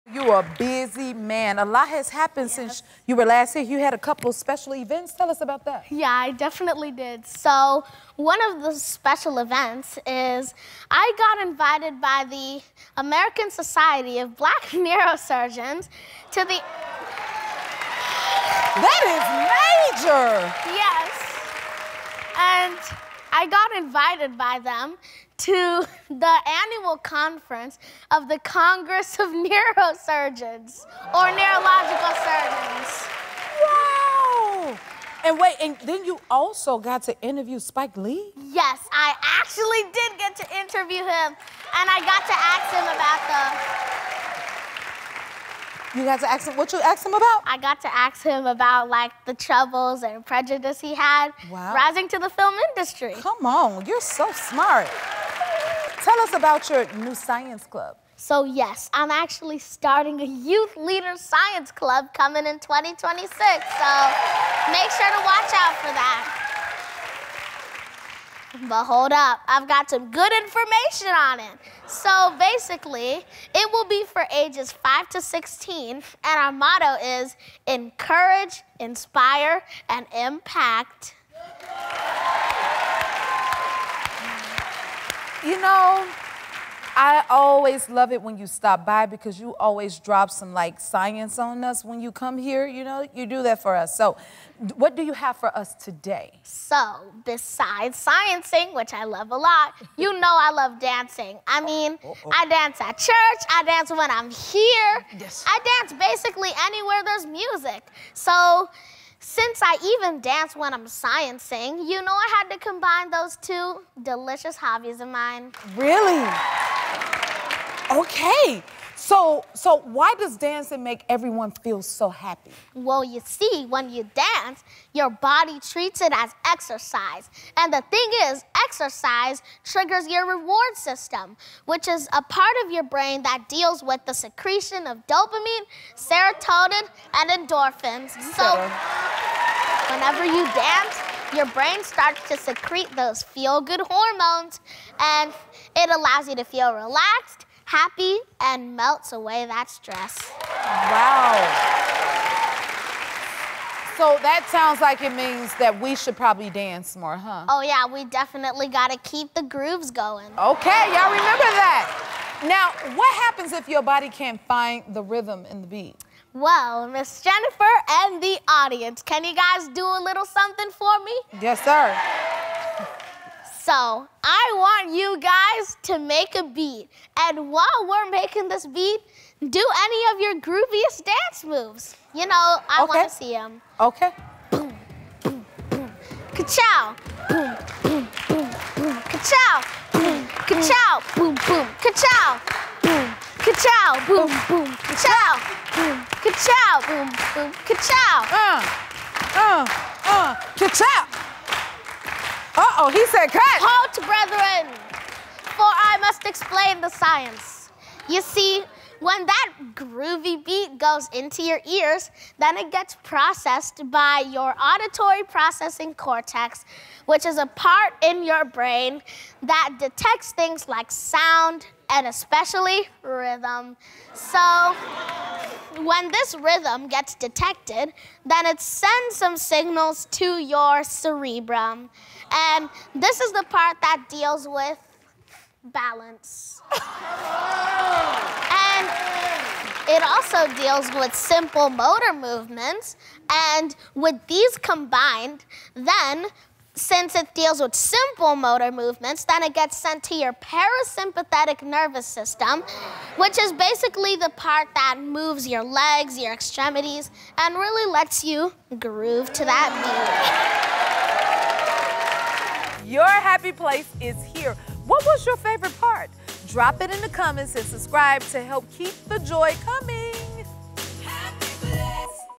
This Is What Happens to Your Brain When You Dance
Science just got groovy! 🕺 Sean the Science Kid is back to break down why dancing makes us feel SO good, from dopamine and endorphins to how your brain actually treats dancing like exercise. Proof that learning can be fun, feel-good, and full of rhythm!